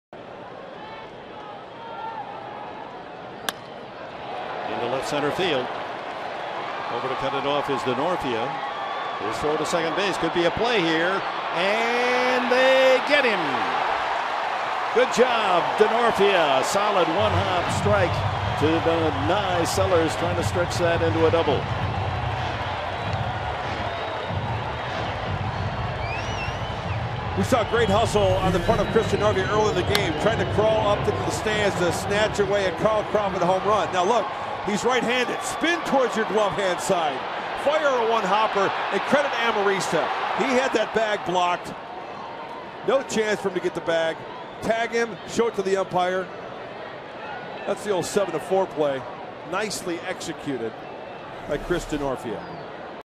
Denorfia throws out Sellers
4/10/13: Chris Denorfia fields a single to left and makes a strong, accurate throw to cut down Justin Sellers trying to stretch for a double